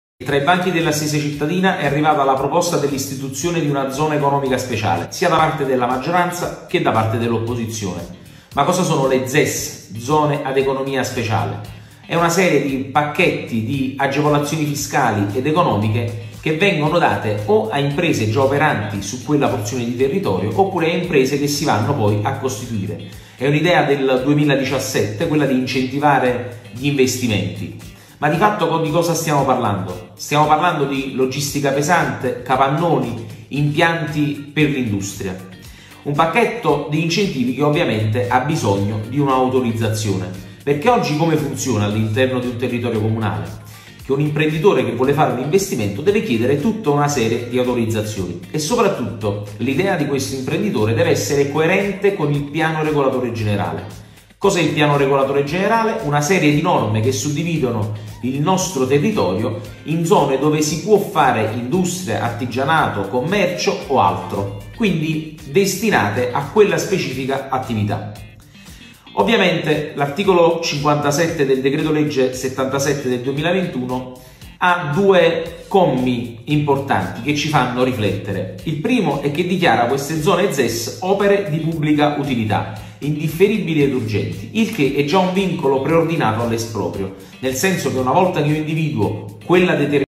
Pozzuoli POZZUOLI ORA DICE NO! Le parole del consigliere di minoranza Riccardo Volpe
Nell’ultimo consiglio comunale è approdata la proposta per far aderire formalmente parti del territorio comunale alla “Zona Economica Speciale”. Si tratta di aree nelle quali, grazie ad un intervento regionale e ad una normativa nazionale, le imprese esistenti o quelle che si andranno ad insediare potranno beneficiare di importanti agevolazioni economiche e finanziarie.

«Sono arrivate due proposte – afferma il consigliere comunale Riccardo Volpe – sia da parte del resto dell’opposizione che da parte della maggioranza. In entrambe venivano indicati pressoché gli stessi luoghi: Porto, Rione Terra, ex Sofer ed in una c’era finanche l’ex convitto Monachelle. Istituita una zona ZES – continua Volpe – il consiglio comunale e la città non hanno più potere decisionale, perché a rilasciare tutte le autorizzazioni è un Commissario straordinario di Governo. La perimetrazione di queste zone è cosa ben più complessa, proprio perché nelle parti di territorio prescelte non vigerà più il nostro Piano Regolatore, andando in deroga a le norme di tutela. Per questo – continua Volpe – pensiamo che luoghi di interesse strategico come ex Sofer, Rione Terra, debbano essere esclusi da un simile ragionamento, che porterebbe tutte le decisioni in altre sedi. Su questo – conclude Volpe – ci batteremo duramente, affinché sia la Città a decidere sul proprio futuro».

I risultati dei lavori gestiti da un Commissario di Governo sono sotto gli occhi di tutti: il ponte mostro, il tunnel di Lucrino e grandi opere. Milioni bruciati per scelte rispetto alle quali la città non ha potuto batter ciglio.

«Si tratterebbe dell’ennesimo ricorso a interventi commissariali – dichiara Raffaele Postiglione, di Pozzuoli ORA! – Ancora una volta le Istituzioni elette dai cittadini verrebbero svuotate. Un vulnus democratico contro il quale ci siamo sempre battuti. Tra l’altro – continua Postiglione – non ci sono esempi virtuosi di aree ZES nel nostro Paese. La creazione di occasioni di lavoro, regolare e con pieni diritti, è senz’altro una priorità per il nostro territorio, ma bisogna procedere mirando ad uno sviluppo armonico e sostenibile dal punto di vista ambientale e sociale. Lo strumento di cui si parla riguarda le aree industriali.

Accostare a queste il Rione Terra o l’ex Sofer, che nulla hanno a che fare con nuovi insediamenti industriali, porterebbe ad una deregulation e ad una estromissione della Città dalle scelte su pezzi strategici del nostro territorio. Il nostro agire politico – conclude Postiglione – sarà sempre orientato alla creazione di occasioni diffuse e mai ad appannaggio di grandi gruppi imprenditoriali o dei cosiddetti “poteri forti” rispetto ai quali sono tutti contro, ma solo a parole. Le zone ZES potrebbero esserne l’ennesima dimostrazione».